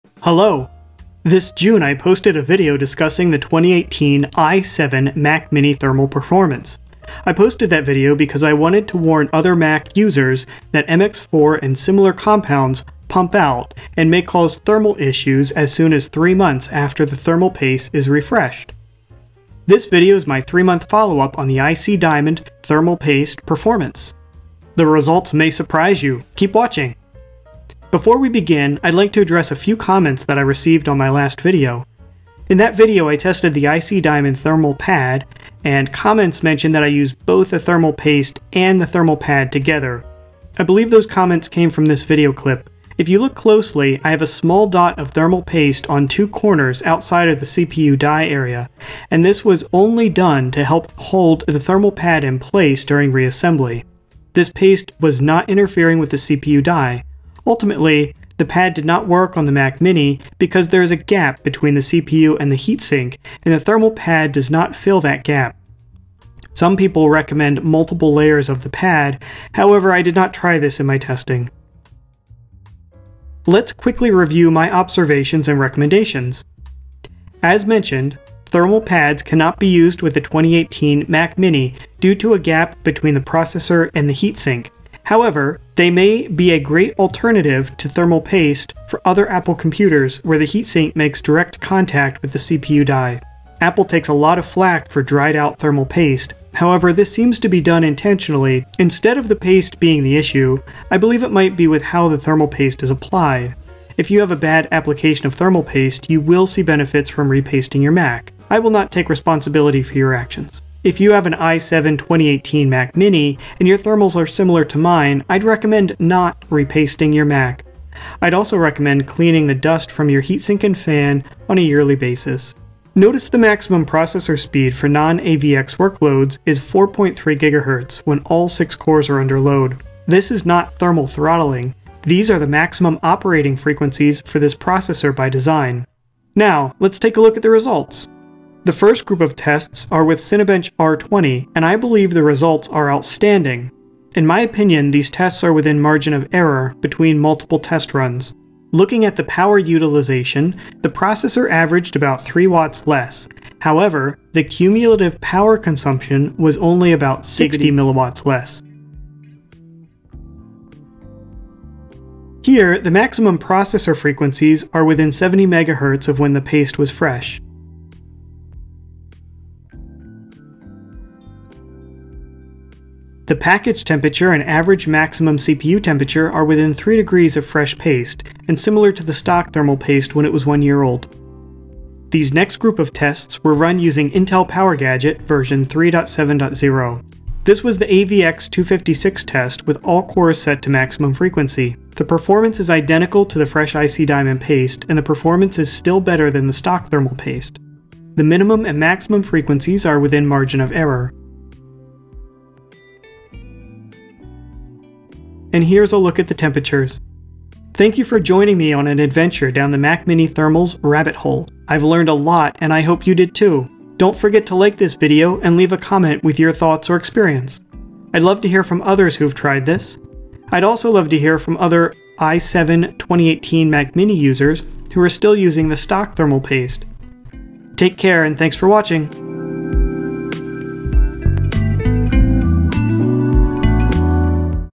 Mac Mini Thermal Issues Follow up
Three months later, how does IC Diamond compare to MX-4?  Sometimes thermal paste can push-out from between the CPU and heatsink causing poor thermal performance.  This video explores my experience with Arctic MX-4 and Innovation Cooling Diamond thermal paste performance after 3 months.

This is a follow-up to my first video: "Replacing the Mac mini Thermal Paste? Watch this first!"

Geekbench 5 Scores, not shown in the video:
Single-Core: 1122
Multi-Core: 5804
OpenCL: 5356
Metal: 5021

2:16 Results